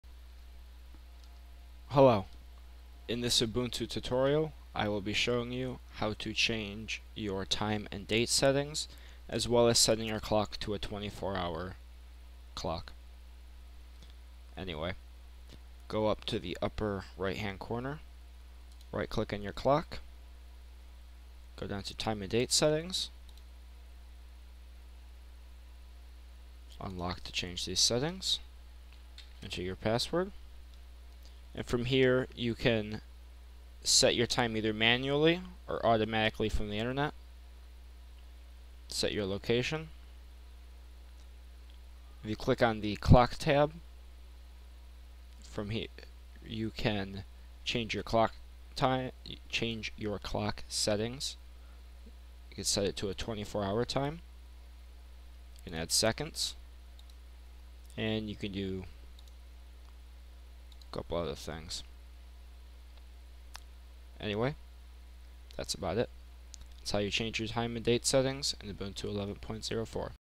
Change Time and Date Settings in Ubuntu 11.04
In this tutorial, I will show you how to change your time and date settings in Ubuntu 11.04